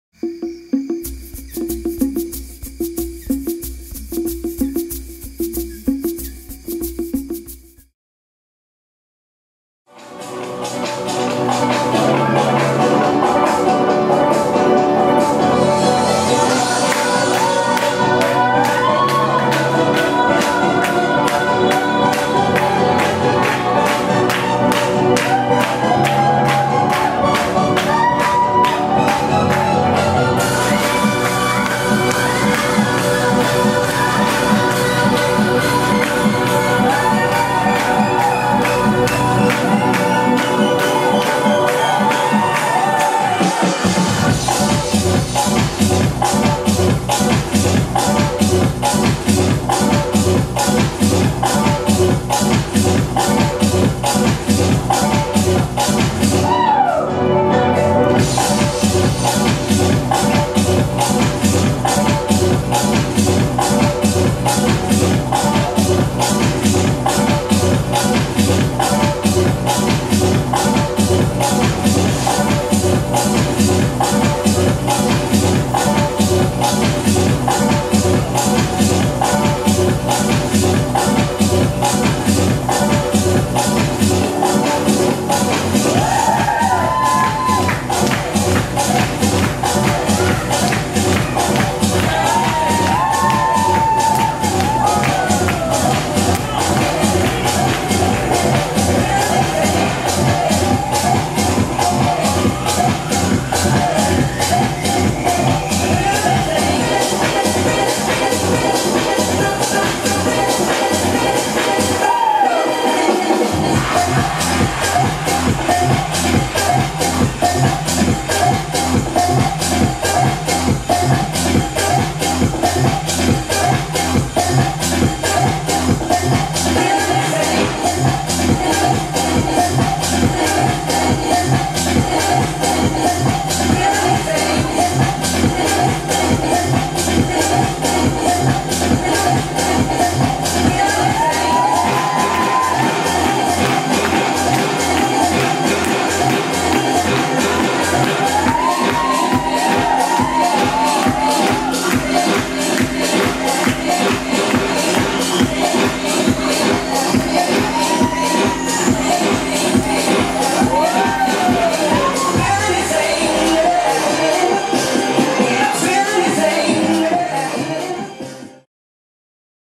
DAKAR 2012 - CARLOS - STEP
DAKAR FITNESS CONVENTION 2012
Cours de Step avec Carlos RAMIREZ